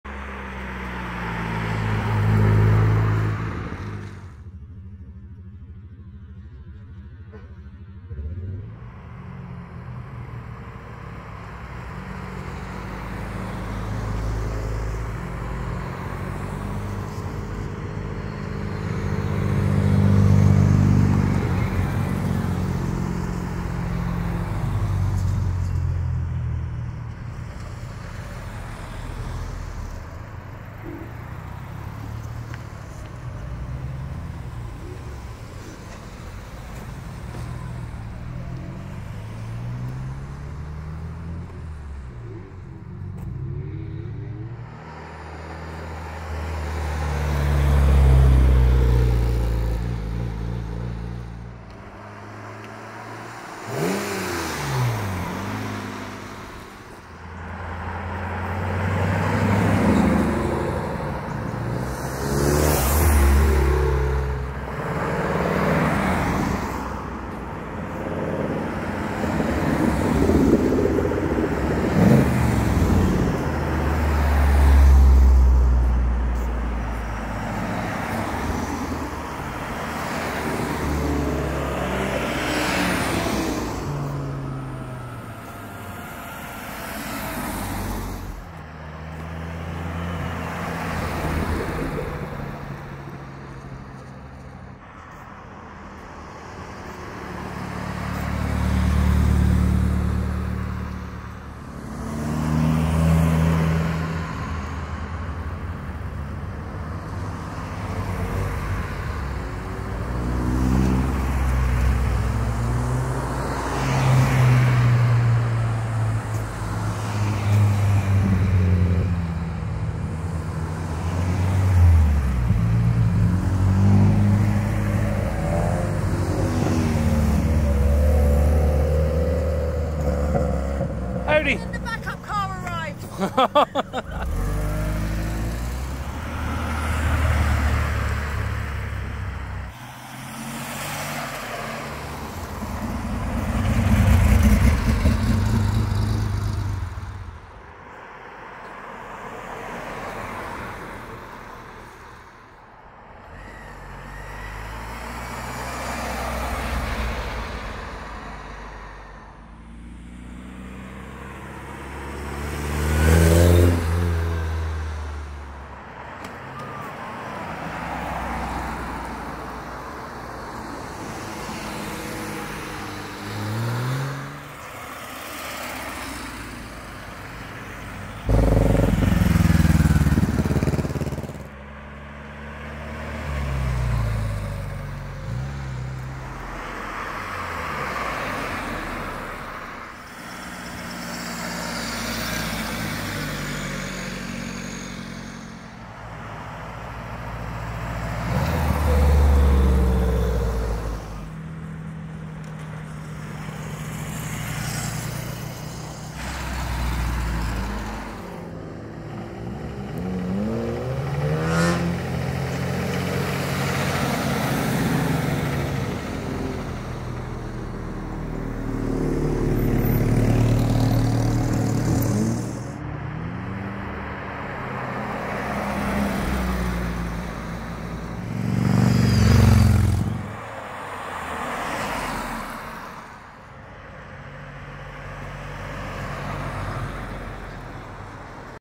06/11/22 - Cars And Coffee North East
A few clips of some of the cars arriving to DKIT for Cars And Coffee.

Video by ArdeeSnapper